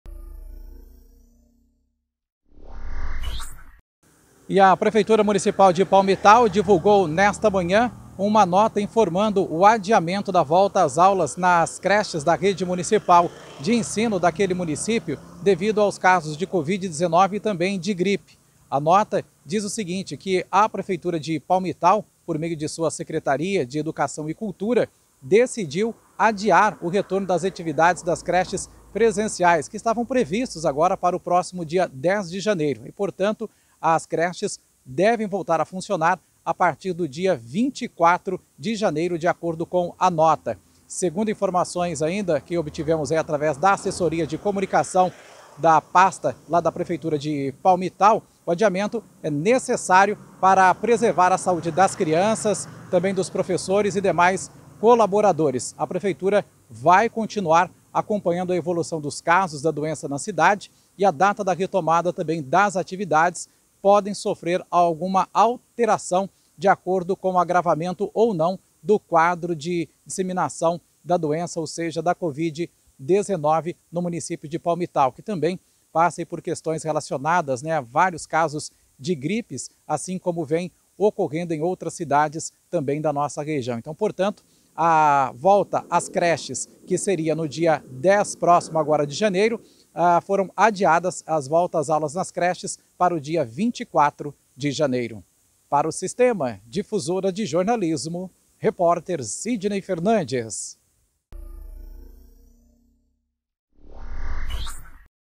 Prefeitura de palmital comunica o adiamento do retorno das creches para o dia 24/01
Prefeitura de palmital comunica o adiamento do retorno das creches para preservar a saúde das crianças e profissionais da educação.